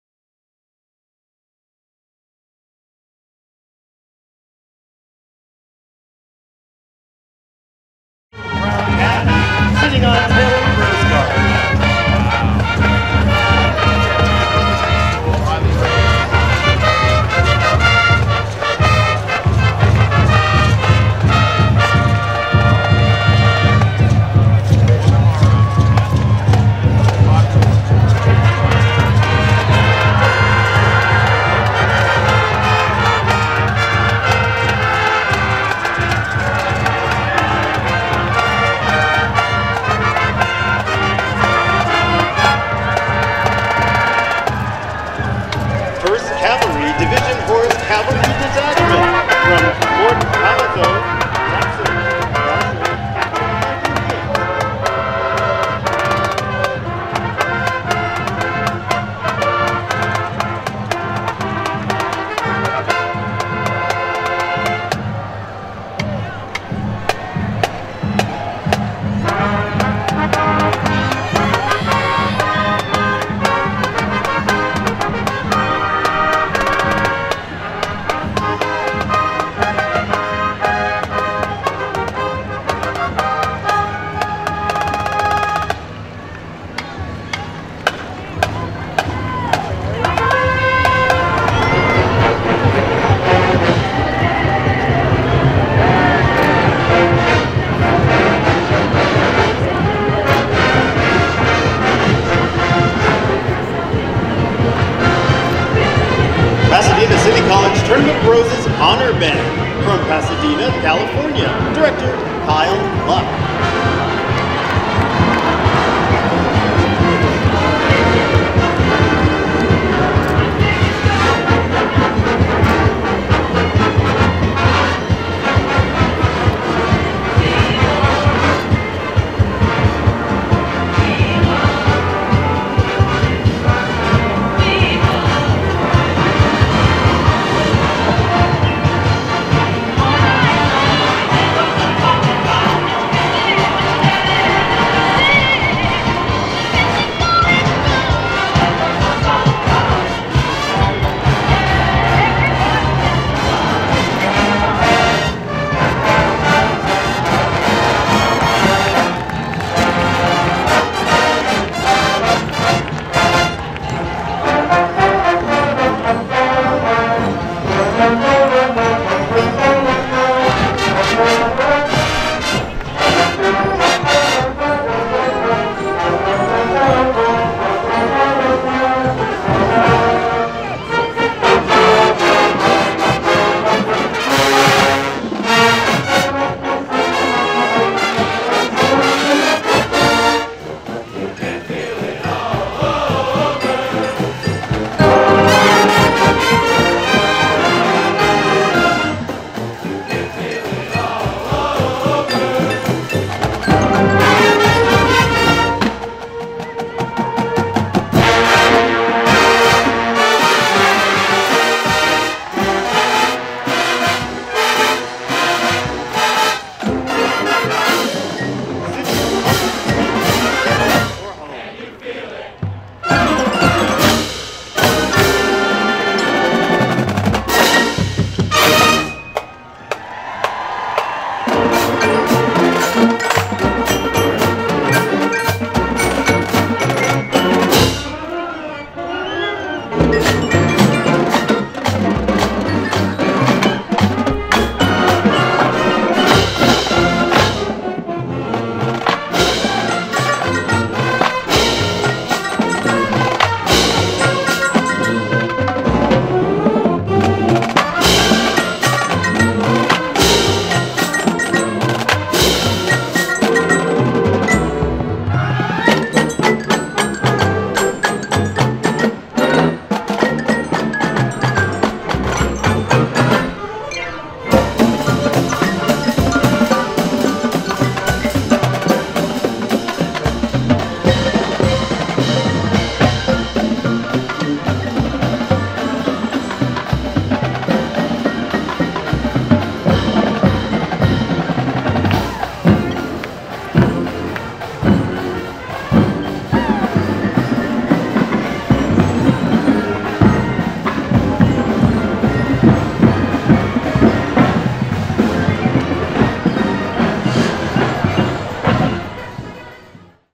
2024 PCC Tournament of Roses Honor Band & Herald Trumpets - 2024 Pasadena Rose Parade
From Pasadena, CA
The 2024 Pasadena City College Tournament of Roses Honor Band & Herald Trumpets performing at the 135th Pasadena Tournament of Roses Parade on Monday, January 1, 2024.